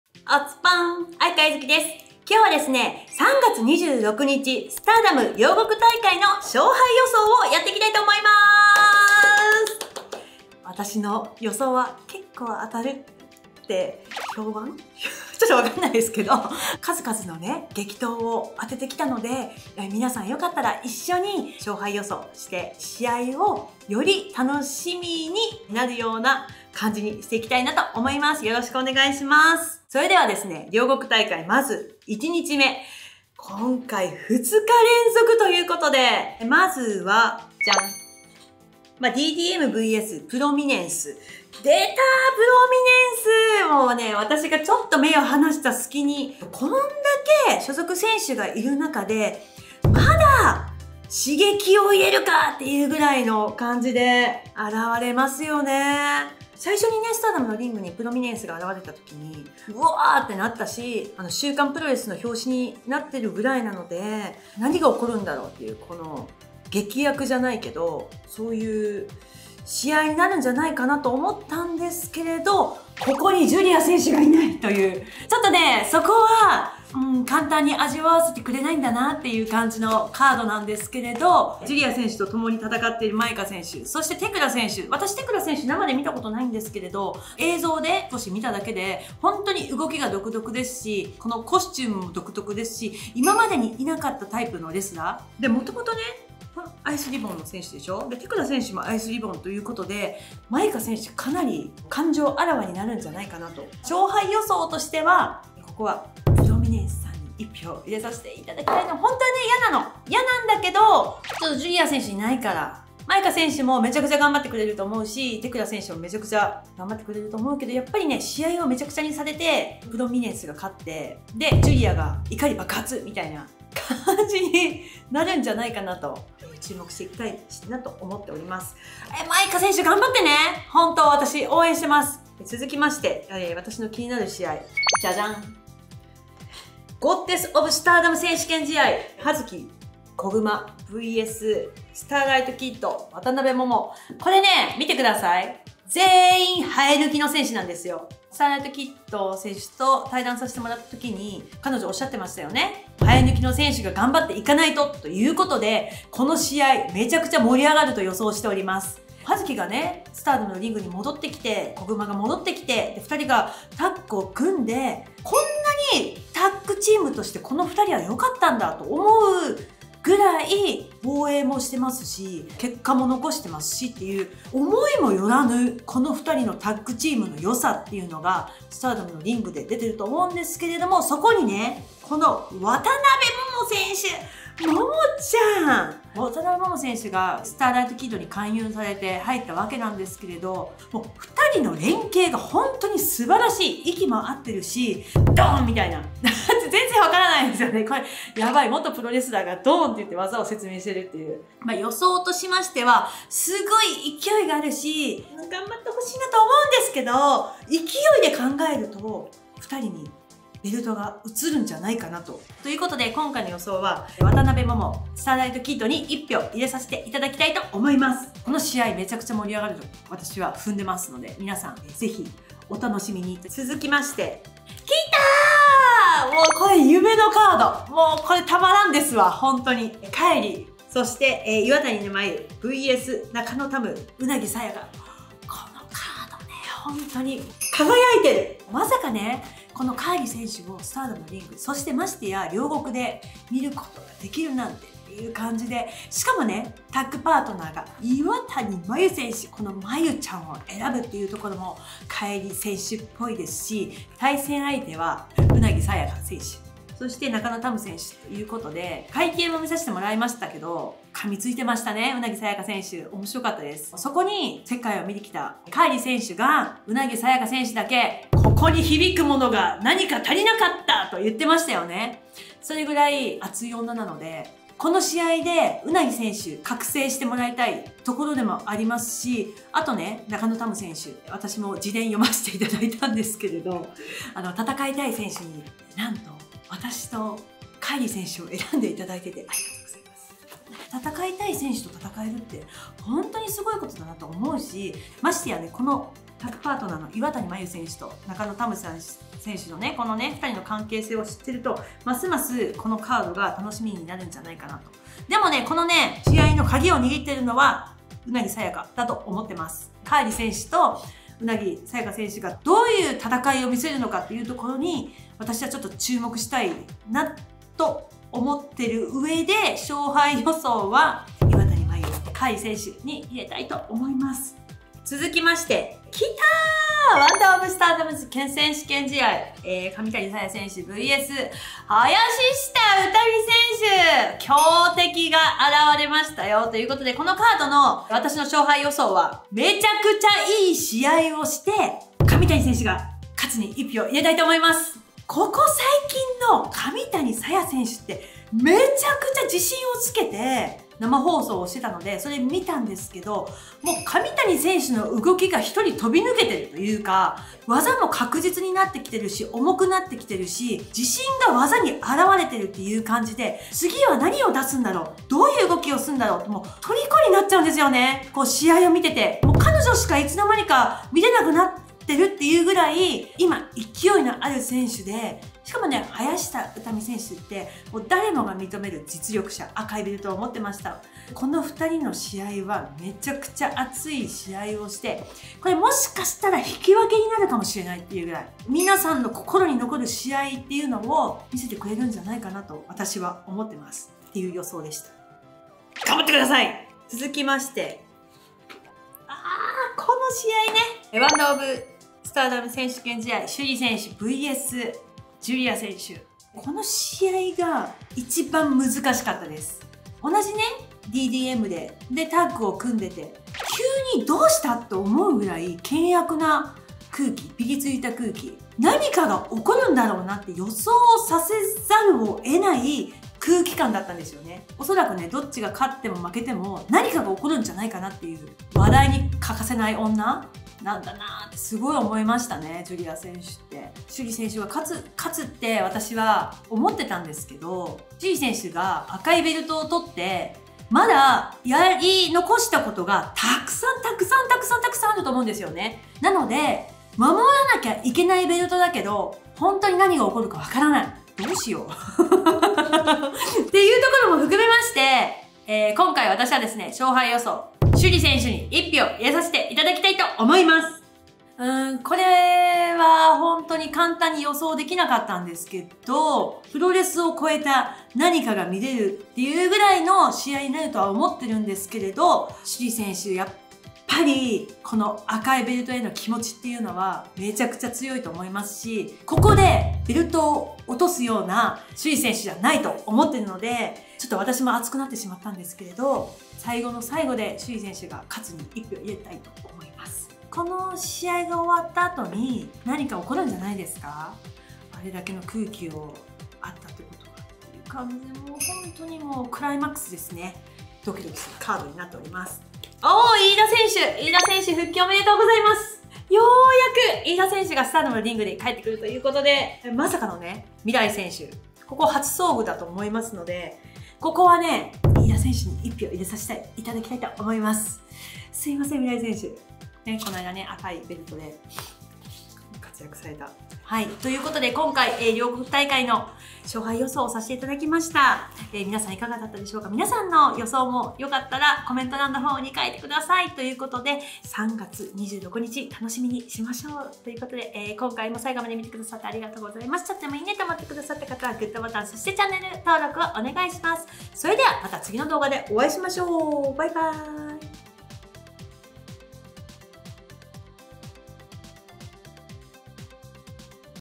【勝敗予想】3.26スターダム両国大会の勝敗予想！朱里vsジュリア・上谷vs林下・KAIRI復帰戦【STARDOM】
おつぽん💛愛川ゆず季です！
今回は、3.26 スターダム両国大会の勝敗予想をやってみました！
【目次】
0:00 3.26スターダム両国大会 勝敗予想
0:35
◆DDMvsプロミネンス
舞華＆テクラ（Donna del Mondo）
vs 世羅りさ＆鈴季すず（プロミネンス）
2:13
◆ゴッデス・オブ・スターダム選手権試合／30分1本勝負
《王者チーム》コグマ＆葉月（STARS／FWC）
vs 《挑戦者》渡辺桃＆スターライト・キッド（大江戸隊／BLACK DESIRE）
3:44
◆海賊王女スターダム復活　スペシャル・タッグマッチ
岩谷麻優（STARS）＆KAIRI
vs 中野たむ＆ウナギ・サヤカ（COSMIC ANGELS）
5:25
◆ワンダー・オブ・スターダム選手権試合／30分1本勝負
《王者》上谷沙弥（Queen's Quest）
vs 《挑戦者》林下詩美（Queen's Quest
6:37
◆ワールド・オブ・スターダム選手権試合／30分1本勝負
《王者》朱里（Donna del Mondo）
vs 《挑戦者》ジュリア（Donna del Mondo）
8:23
◆黄金世代シングルバウト
飯田沙耶（STARS）
vs ＭＩＲＡＩ（Donna del Mondo）
8:44 ED
■愛川ゆず季 / Yuzuki Aikawa